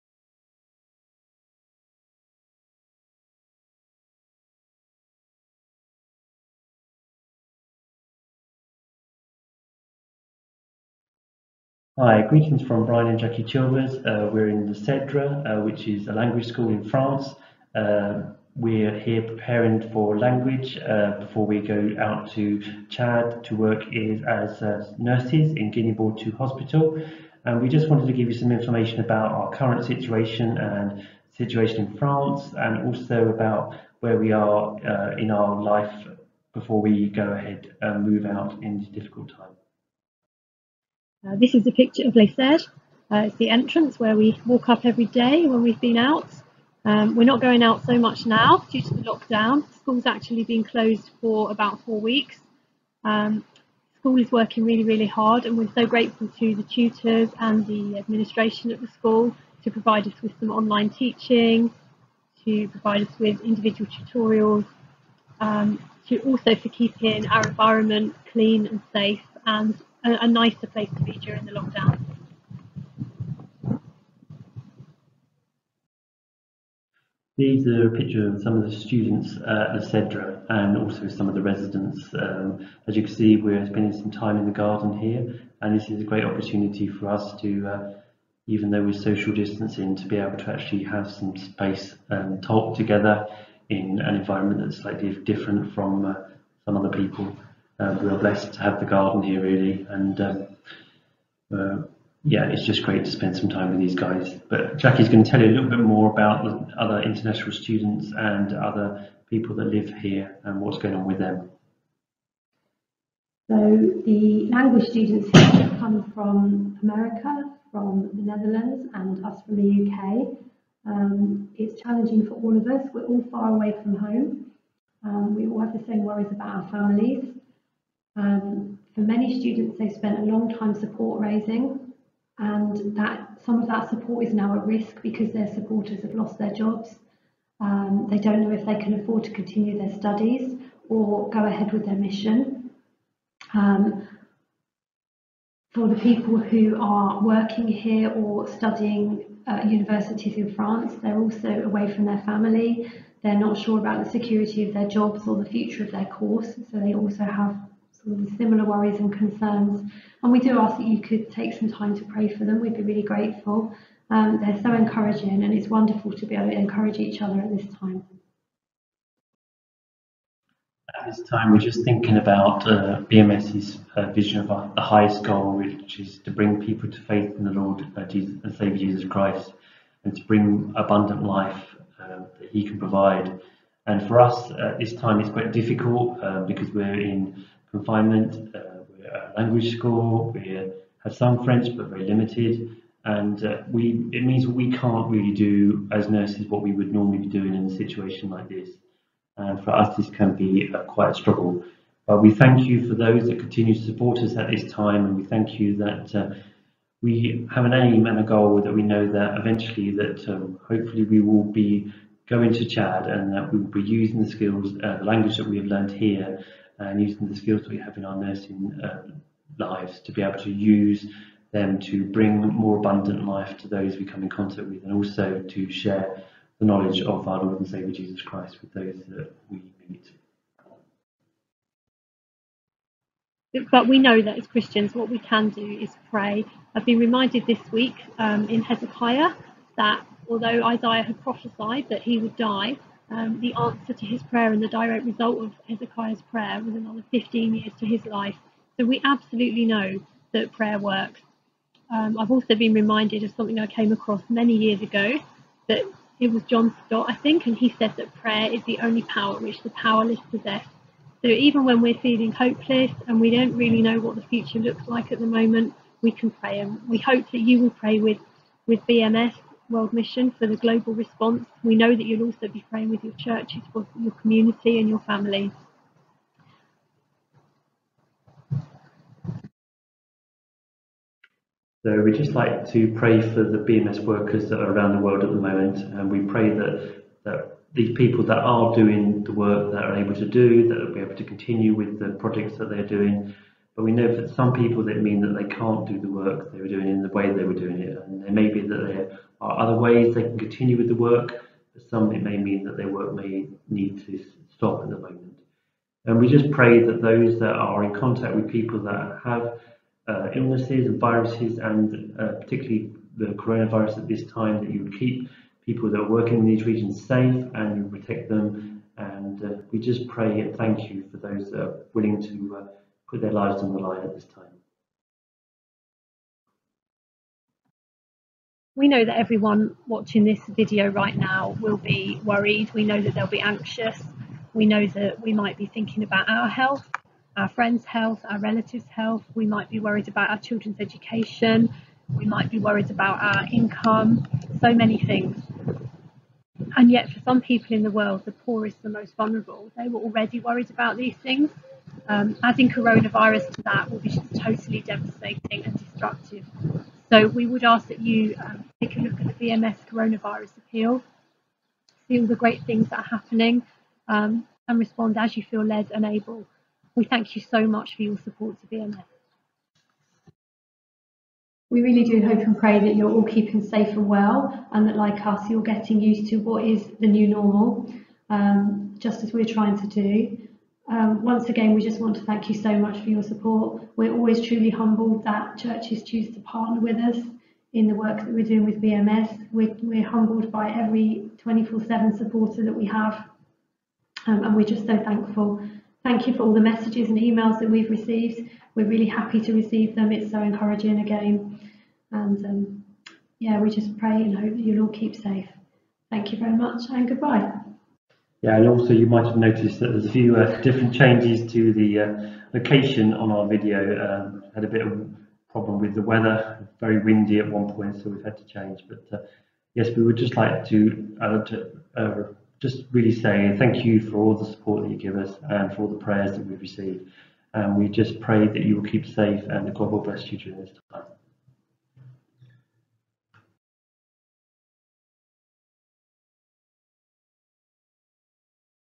Chilvers' Update
Our BMS World Mission partners, Brian and Jackie Chilvers, update us on their latest news from 'lockdown' in Paris